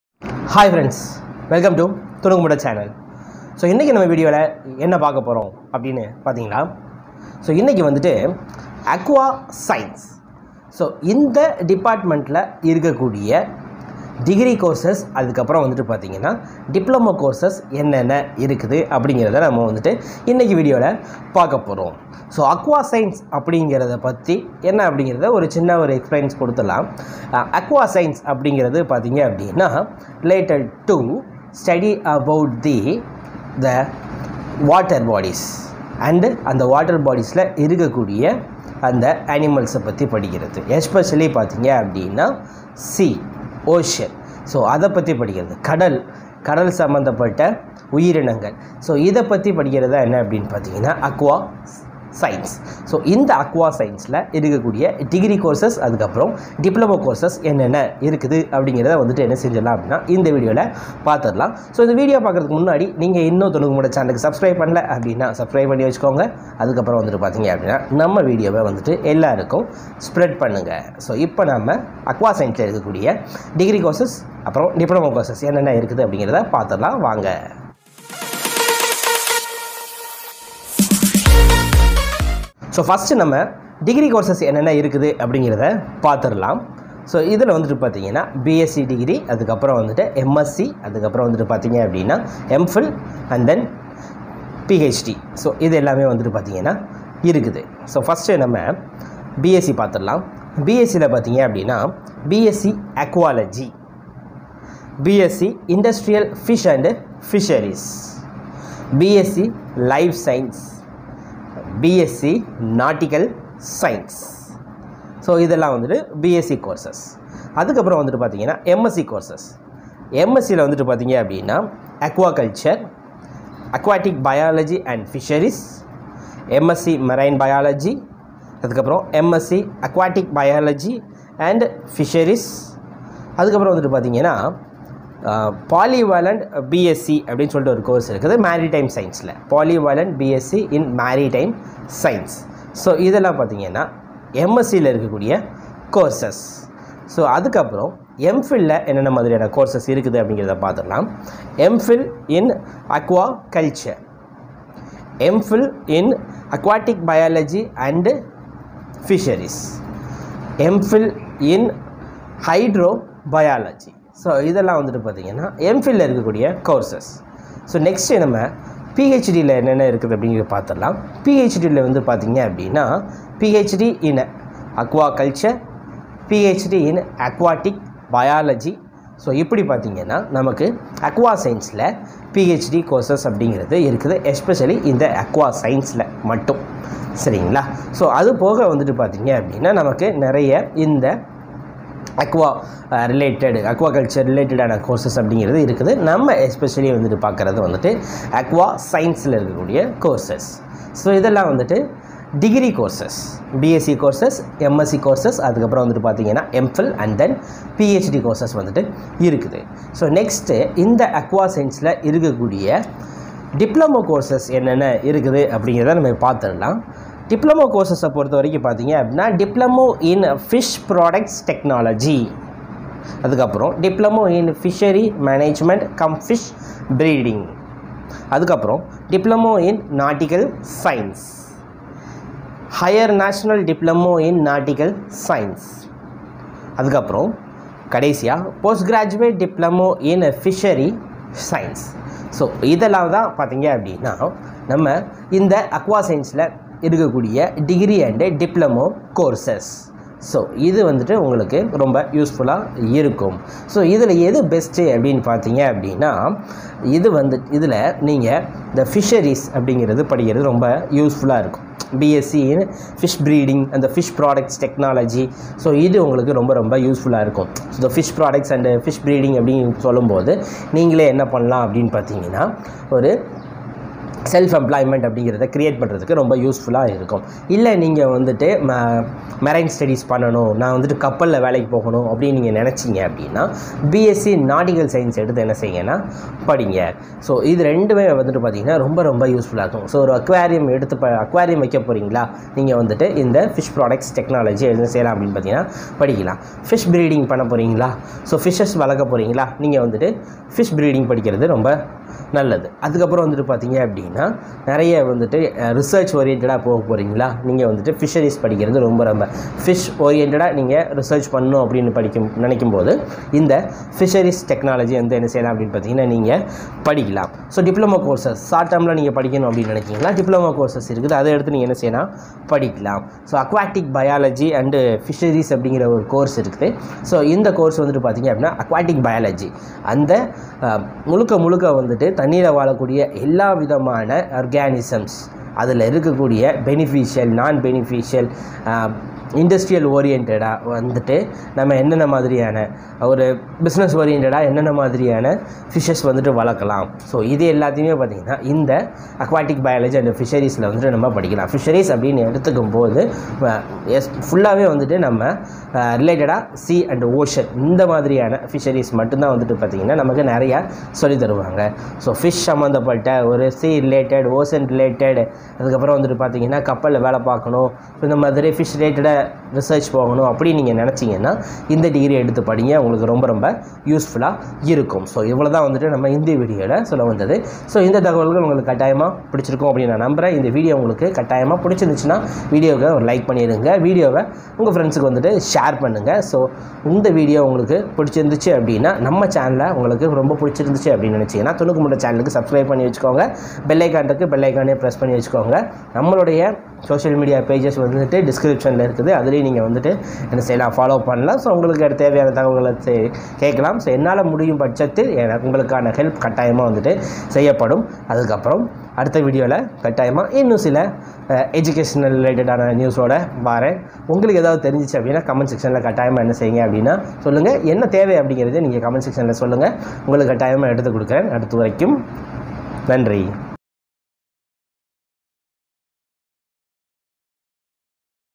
Degree |Diploma |Courses in Aquascience | Bsc | Msc| PhD
This video gives you details about the degree courses and diploma courses in Aquascience department. It is very useful to the students who search for the best degrees for their career.